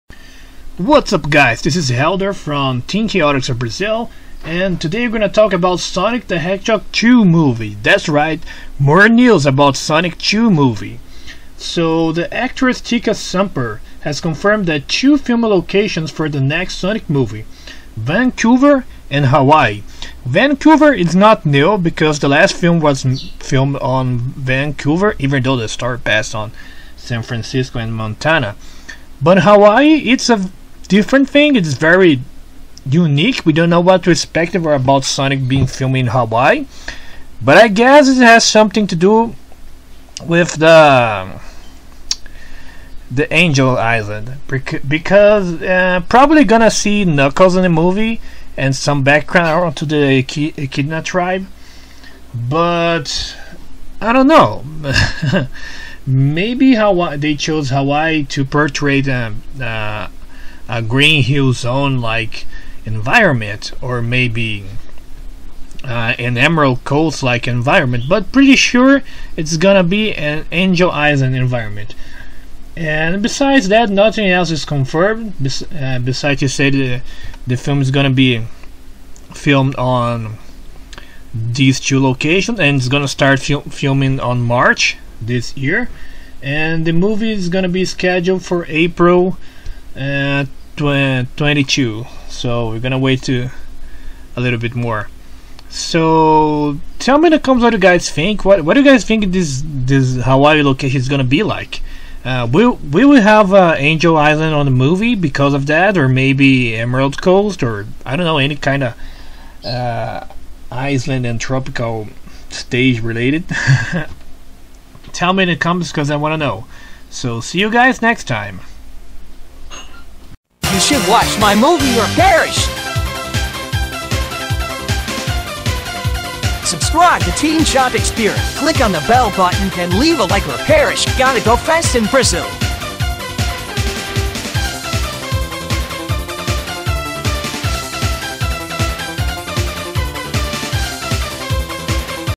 Sonic 2 Movie news Sonic the Hedgehog in Hawaii
Sonic 2 Movie news, Sonic the Hedgehog movie is going to be filmed in hawaii
Big Fishing Quest Fan Game.

Confira a mensagem da SEGA sobre monetização do Youtube:

Customer Service"